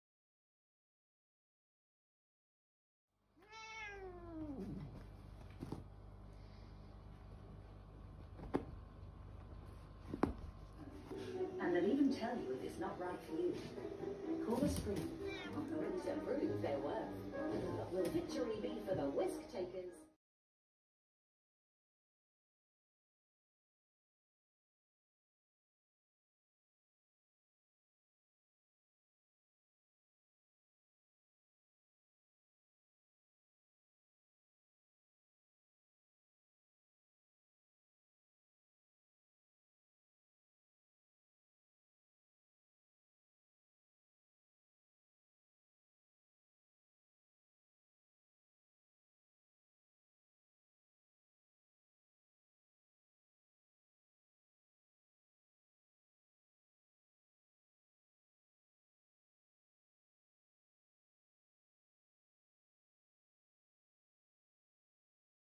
Afghan Hound Puppy & Siamase Cat
Our 4 months old Afghan Hound puppy Shirley and our 9 years old Siamase cat Pipa are becoming more friendly to each other.